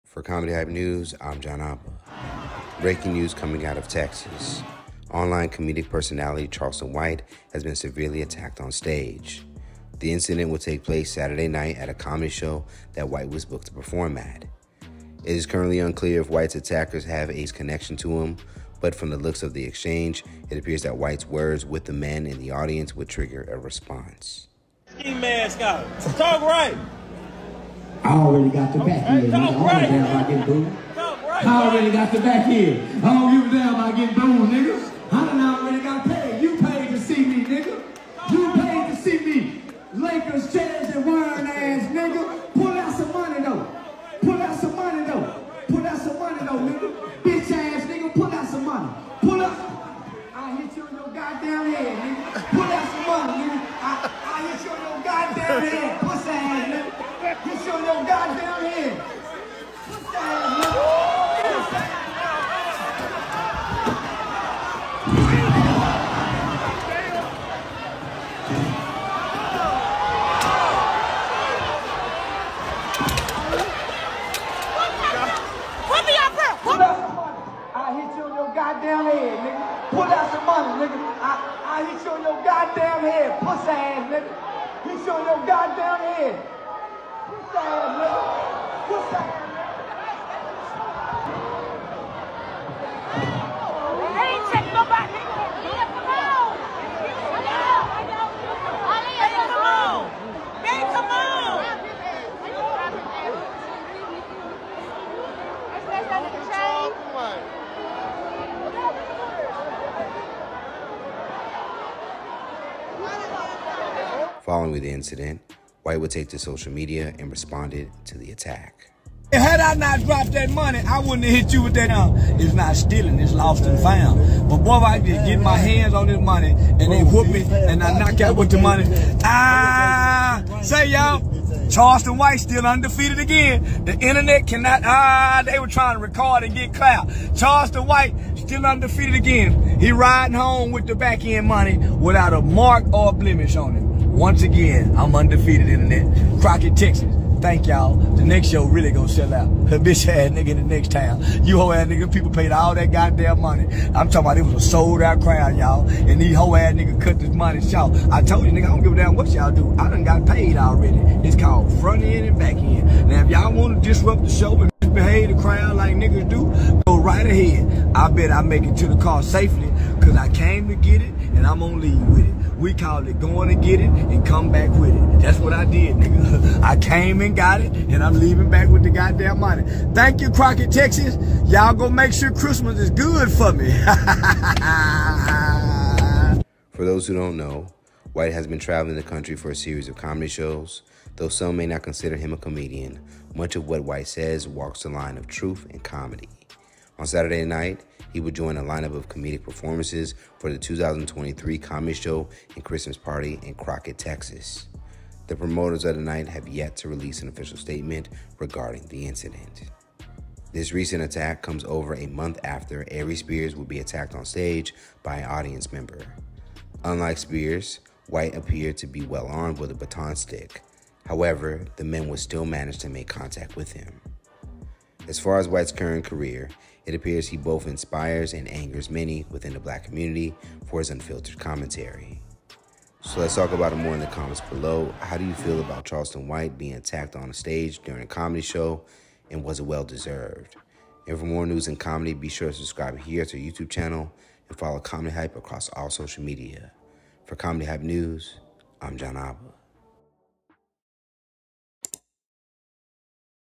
Charleston White Attacked On Stage, Responds To Jumping - CH News
- Online commentator and personality Charleston White has been attacked on stage during a recent comedy show in Crockett, Texas. According to footage, things would get heated between White and a unidentified mad in the audience which would spark a fight on stage. Immediately following the attack, White would take to social media to let people know that he was ok. Instagram: @ComedyHype & Twitter: @ComedyHype_